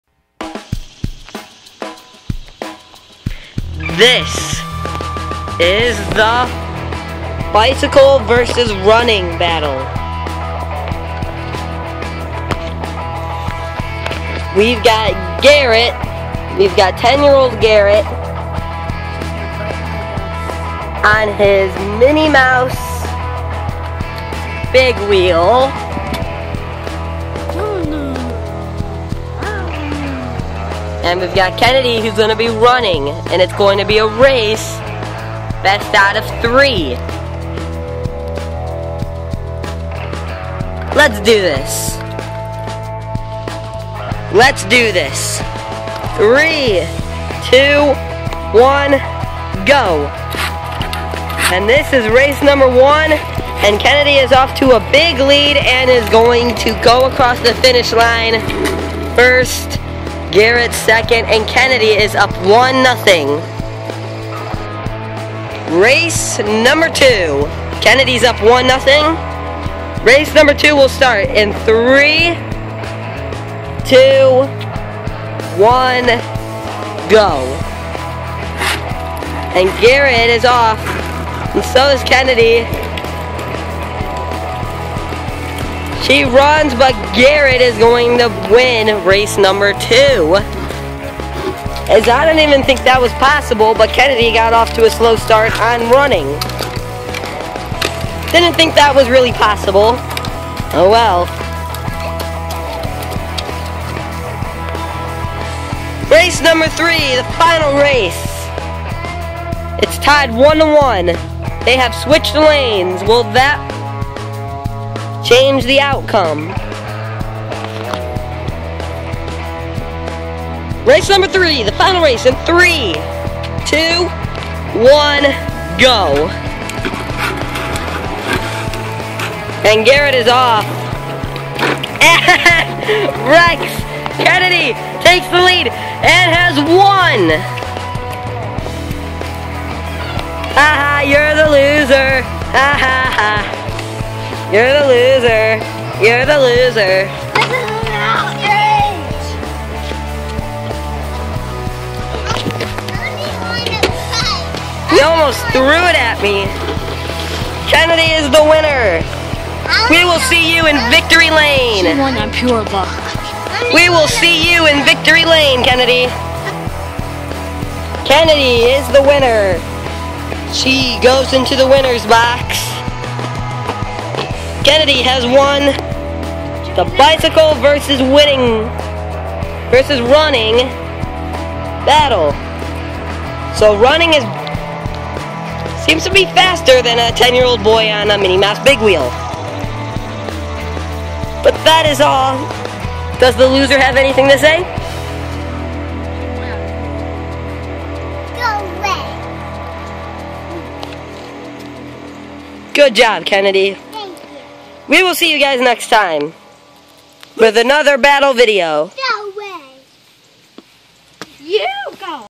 The bike vs running battle!!
Will feet beat a 10 year old on a big wheel? Find out!